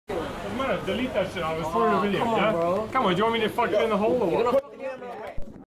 Aza getting molested in subway
11pm saturday night,we entered subway and aza was  recording a random group of males. One of the males confronts aza and tells him delete that shit u want me to fuk u in the hole or what?